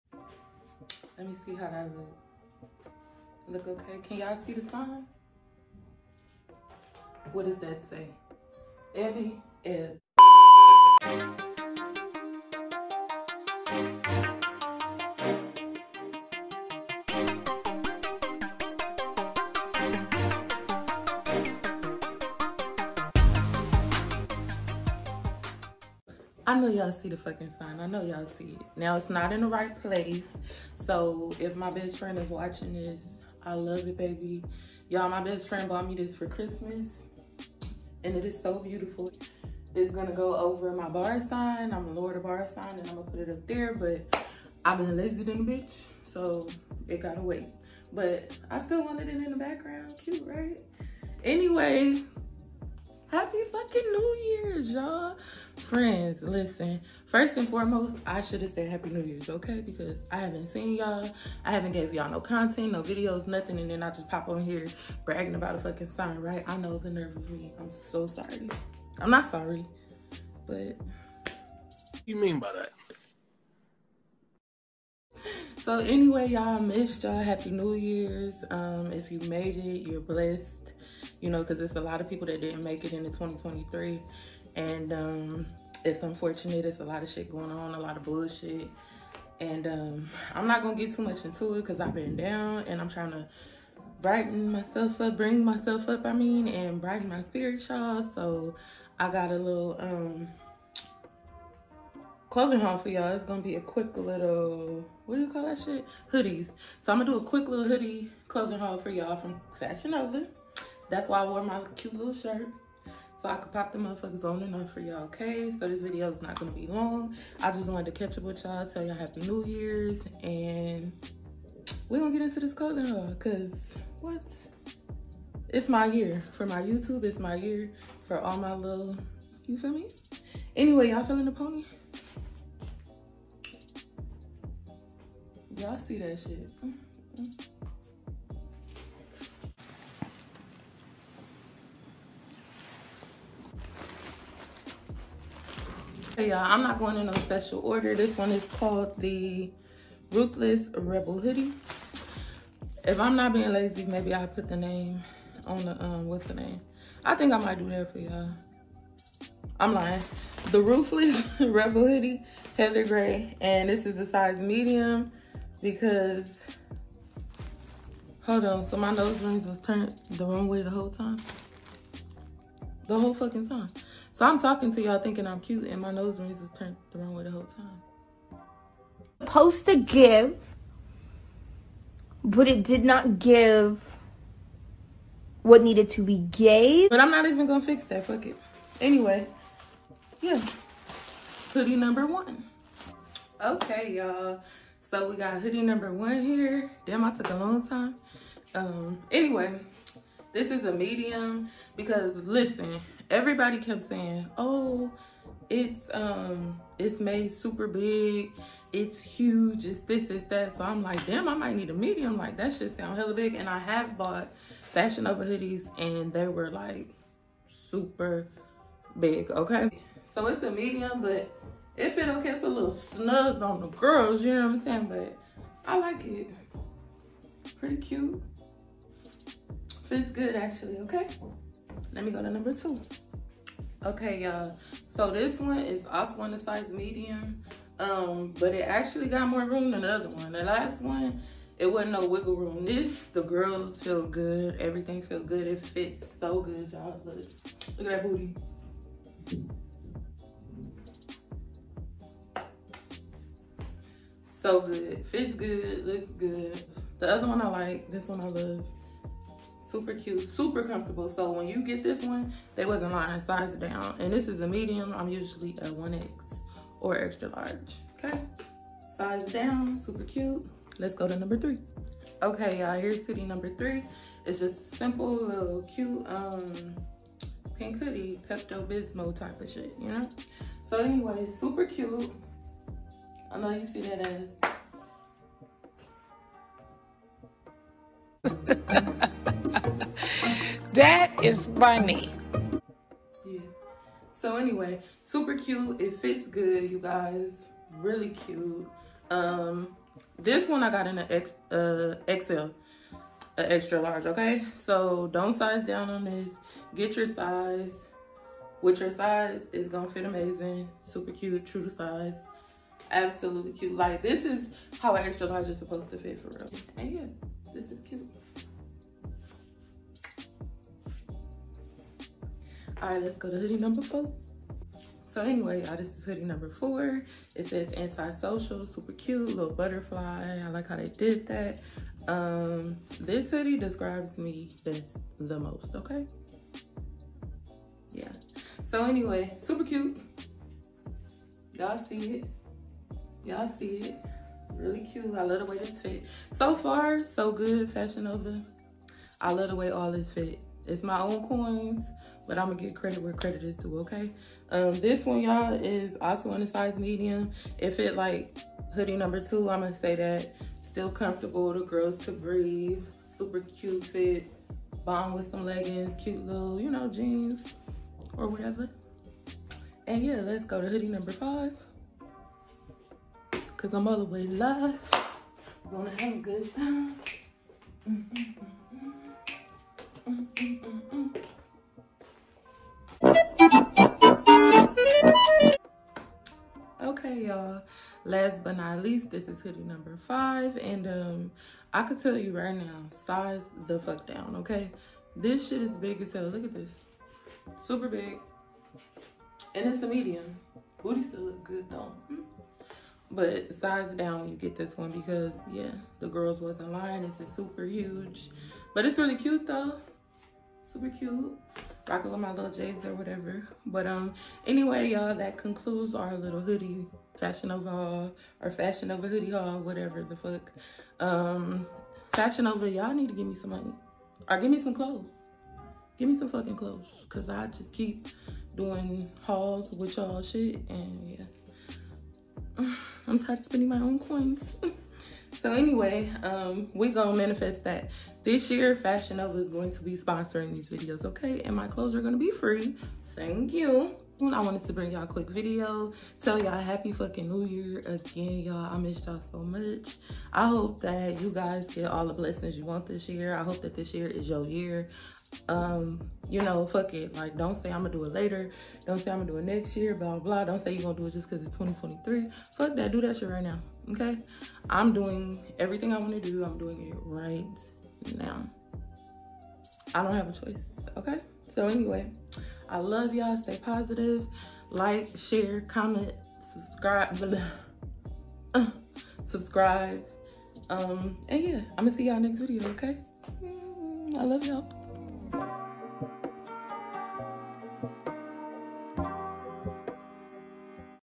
Happy New Year! Quick hoodie haul from Fashion Nova!
Happy New Years freens! I’m late with my first video of the year but hey I’m back 😩🫶🏾…I missed y’all 🥺